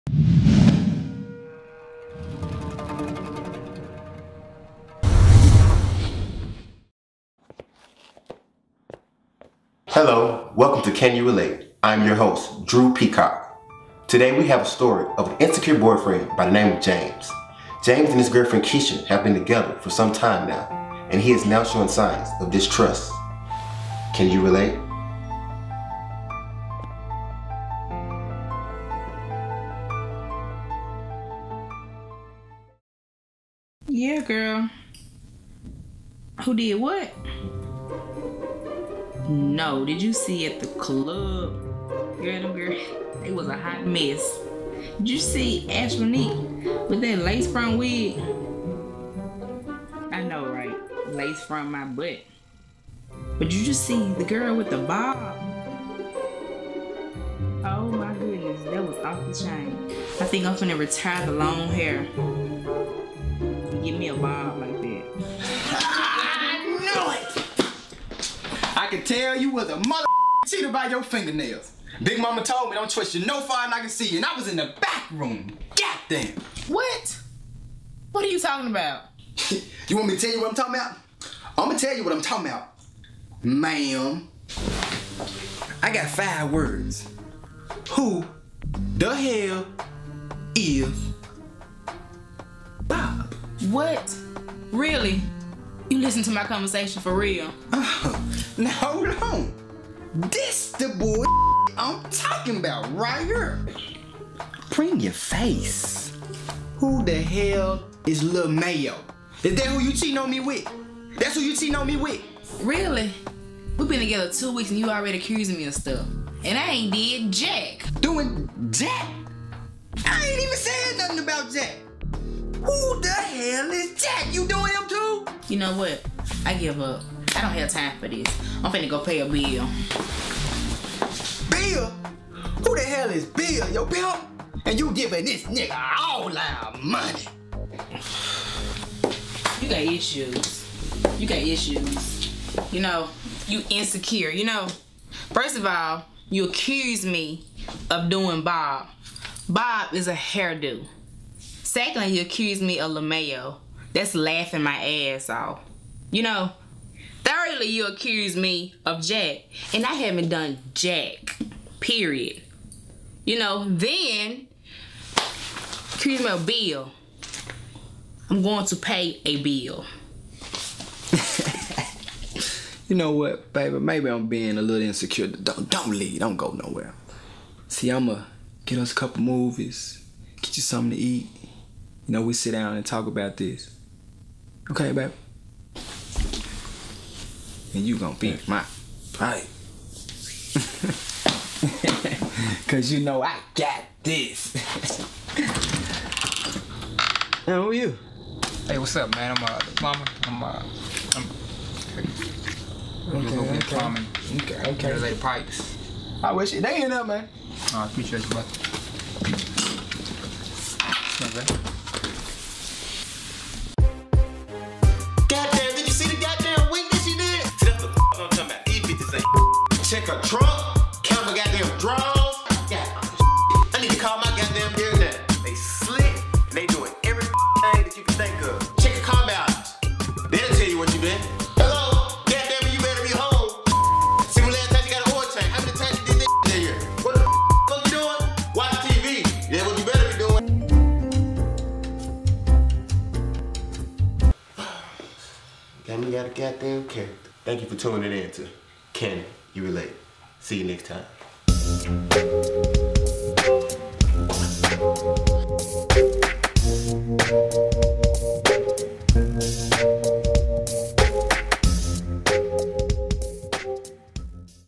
Insecure Boyfriend (Can You Relate)
James is having trust issues with his girlfriend. He believes his girlfriend, Keisha, is cheating on him.